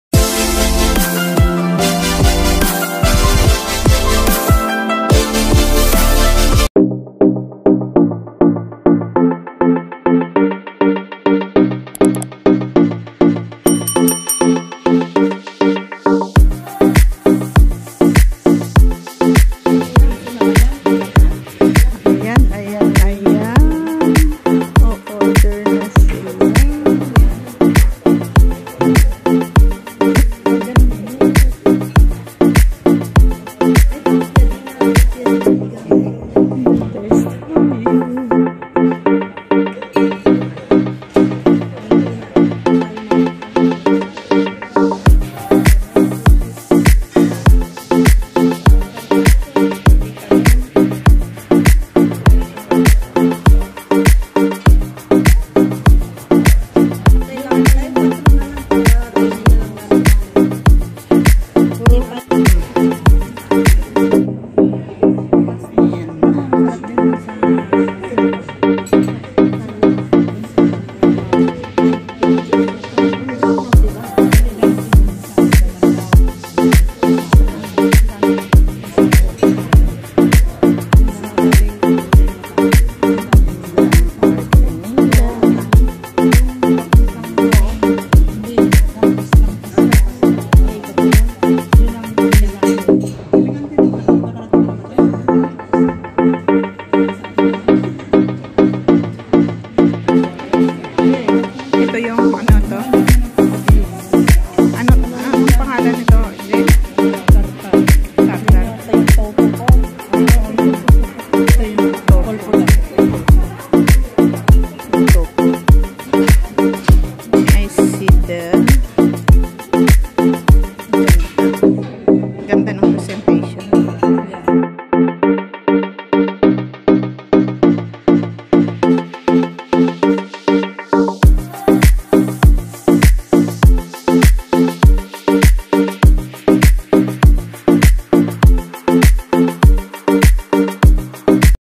WOMEN'S DAY CELEBRATION || @VitaRivas - with friends
A women's ( Friends) date at Lucca, Italy. The last time we are together is February 2020 during my Birthday, I thank you friendster for this time of togetherness once again.